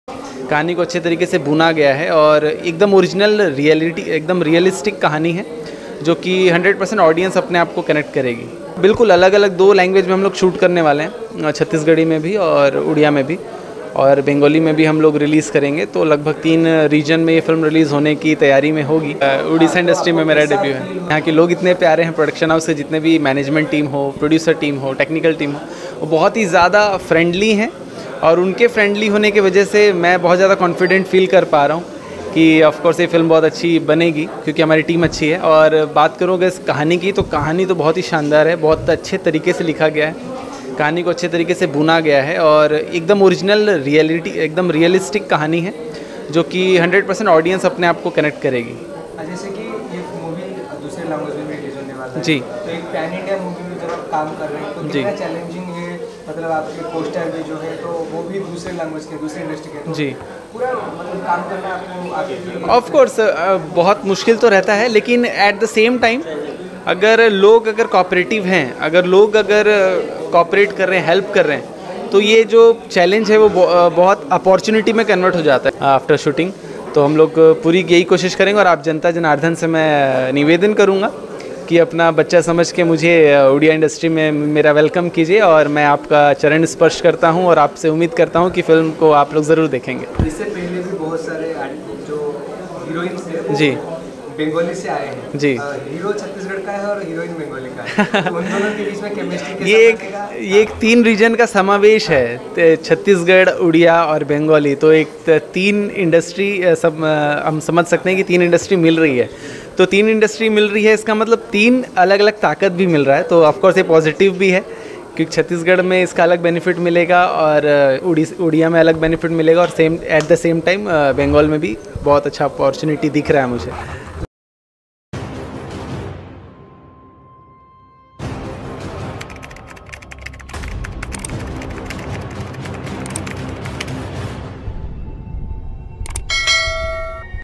ଓଡିଶା ସିନେପ୍ରେମୀ ଦର୍ଶକ ଙ୍କୁ ଅନୁରୋଧ କଲେ ଛତିଶଗଡ଼ିଆ ହିରୋ || odiafilm actor viral statement
ଓଡିଶା ସିନେପ୍ରେମୀ ଦର୍ଶକ ଙ୍କୁ ଅନୁରୋଧ କଲେ ଛତିଶଗଡ଼ିଆ ହିରୋ m odiafilm actor viral statement